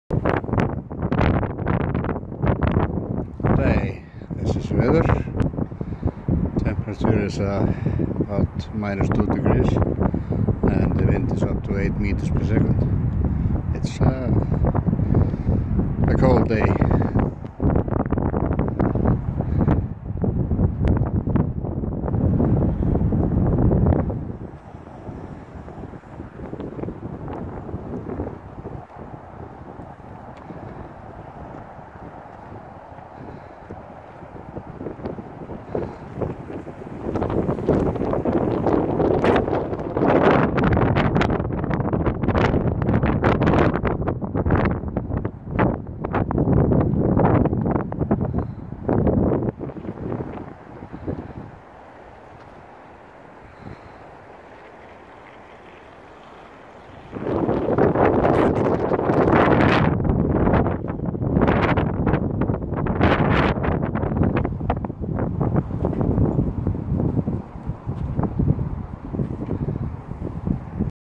Weather
Weather @Iceland December 02 @ 15:55. -2°C, 8 m/s. Kópavogur Reykjavík Capital Area.